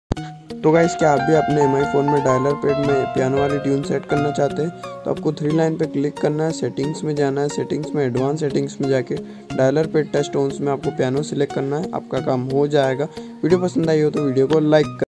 How to add Piano tone in dialerpad of android phone || add piano tone in mi phones
Hello dosto to aaj ki video me hamne apko btaya he ki ap apne mi ke phone me piano vali dialpad tone kese set kr skte h
1. How to enable piano dialerpad tones in mi phones
5. how to customise dialerpad tones in Android